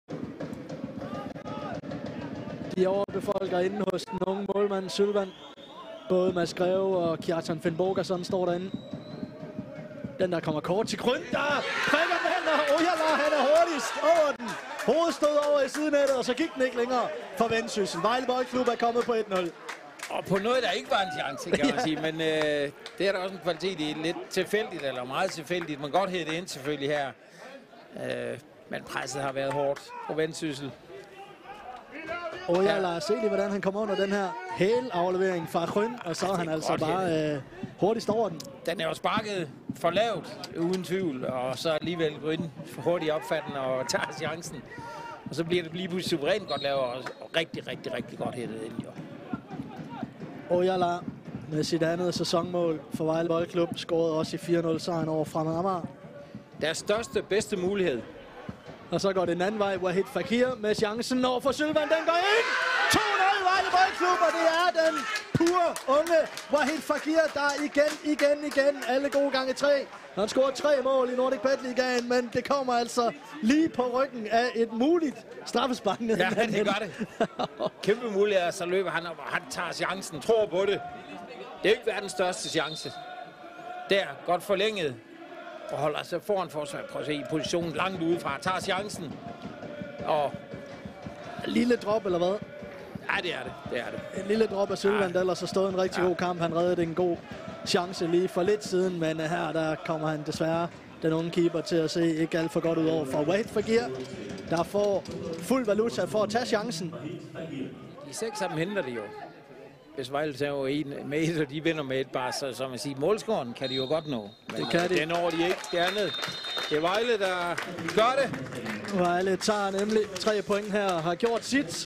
Vendsyssel FF - VB 0-2 (10/07-2020)
Målene fra NordicBet LIGA-kampen mellem Vendsyssel FF og Vejle Boldklub på Nord Energi Arena, Hjørring, fredag den 10. juli 2020.